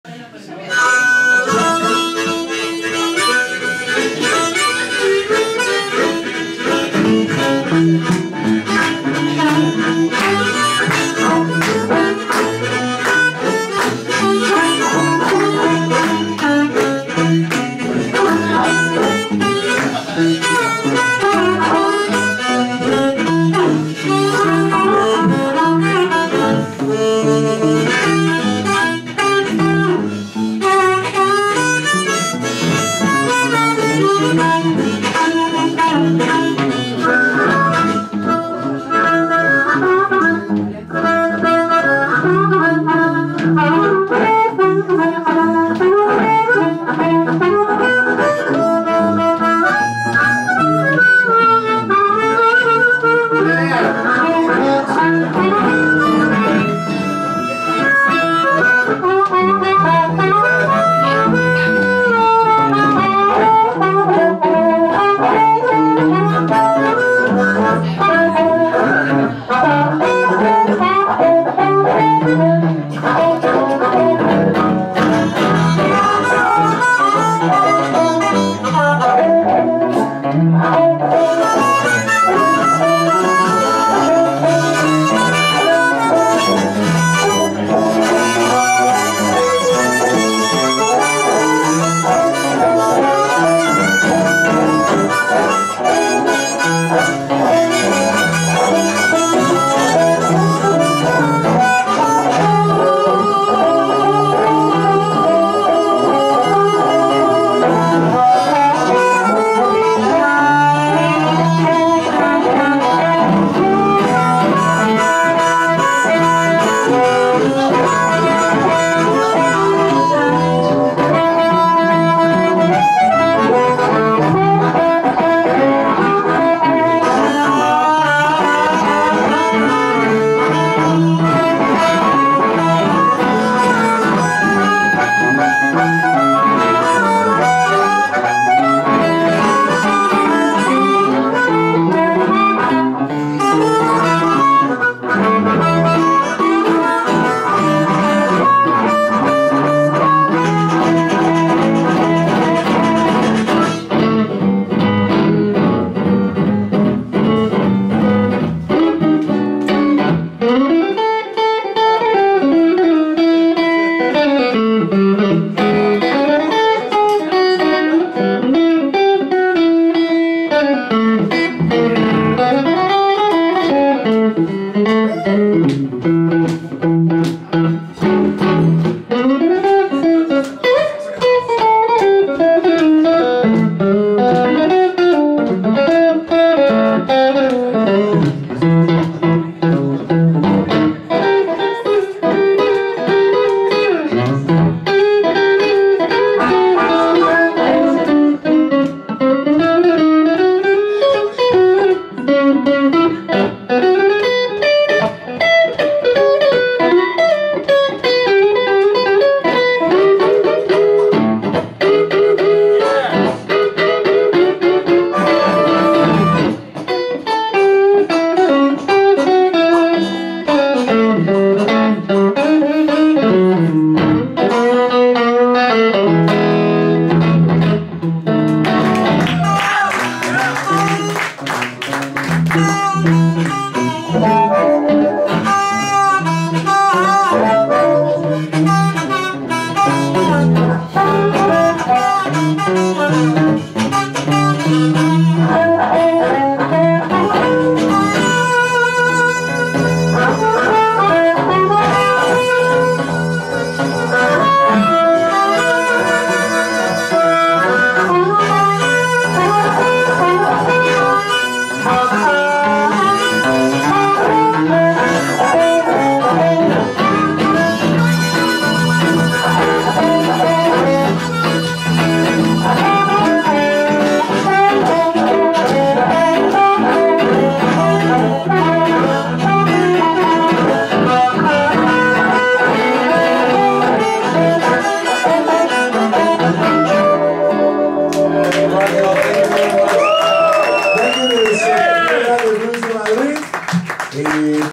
Josh Fulero, Greg Izor & Emilio Arsuaga
SBM - Sunday's Roots Blues Sessions, Taberna Alabanda, domingo 16 abril 2023. (Filmado por Eugenio Moirón)
Josh Fulero (guitarra, voz), Greg Izor (voz, armónica, guitarra), Emilio Arsuaga (voz, armónica, guitarra)